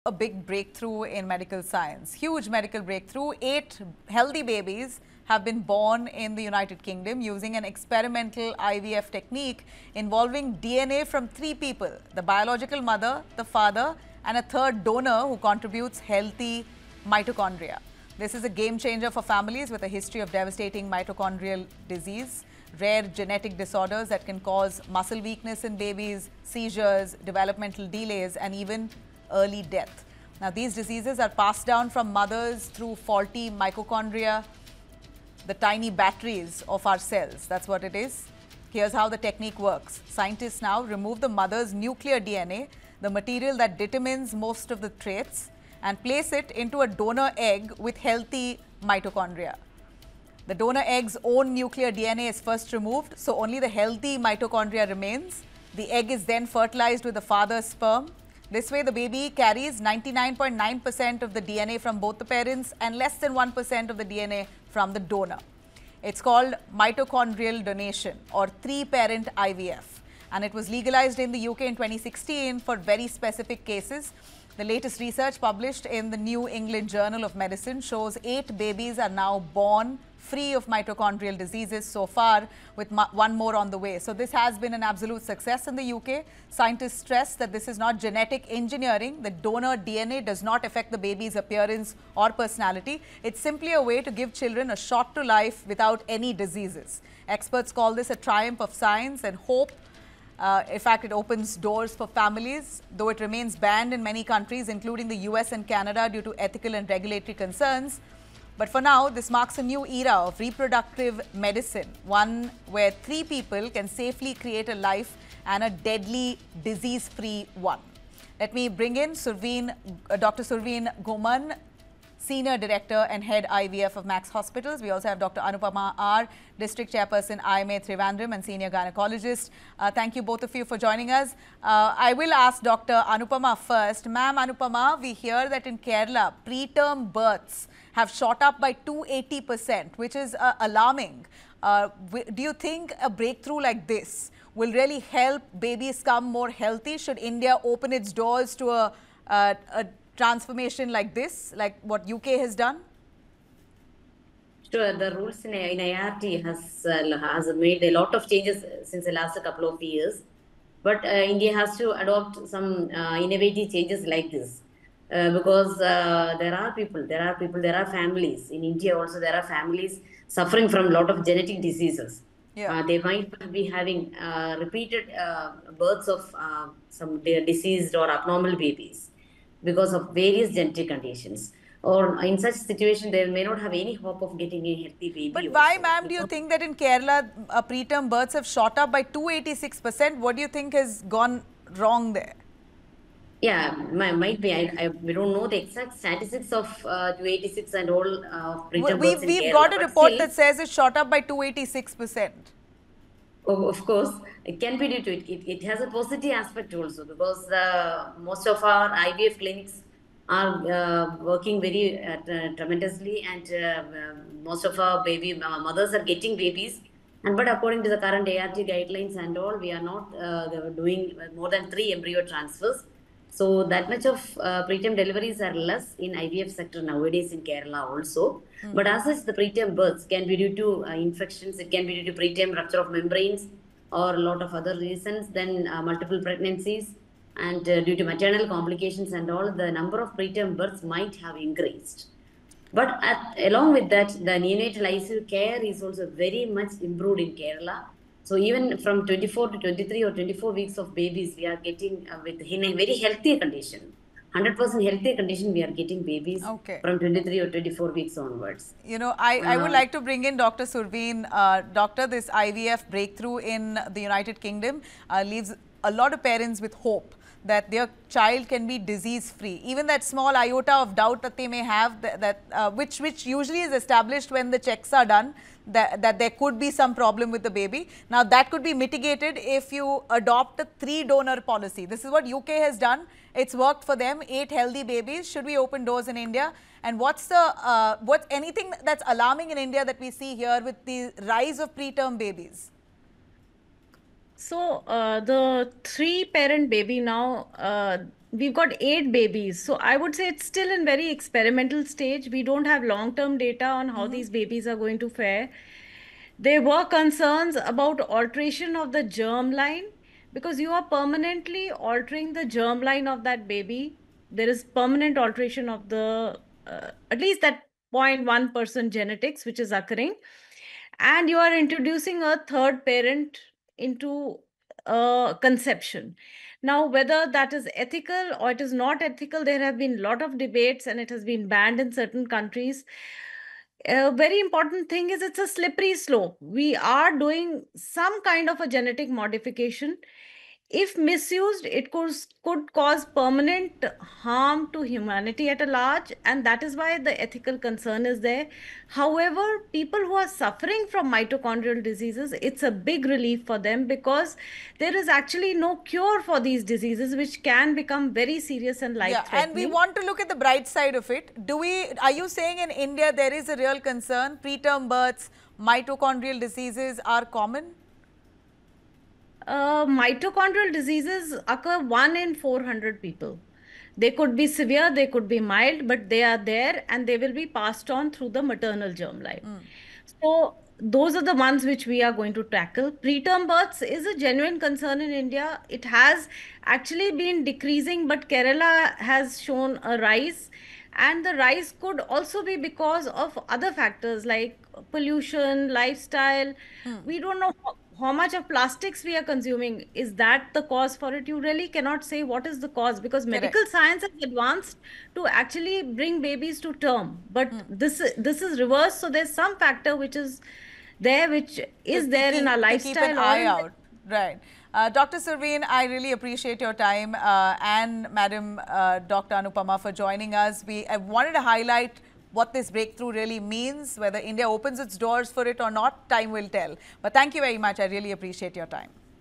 Three-Parent IVF Breakthrough | Eight Healthy Babies Born Disease-Free in UK
In a revolutionary advance in reproductive medicine, eight healthy babies have been born in the UK using a pioneering IVF technique involving DNA from three people — the biological parents and a mitochondrial donor.

This groundbreaking method, known as mitochondrial donation or three-parent IVF, offers hope to families affected by devastating mitochondrial diseases. These conditions, inherited from the mother’s faulty mitochondria, can lead to severe health issues and early death.

✔️ How does it work? Scientists transplant the mother's nuclear DNA into a donor egg with healthy mitochondria, then fertilize it with the father’s sperm. The resulting child carries 99.9% DNA from the parents, and a sliver from the donor — effectively bypassing the genetic defect.

The technique was legalized in the UK in 2016 for specific cases. Now, with eight children born disease-free and one on the way, experts are calling it a triumph of science.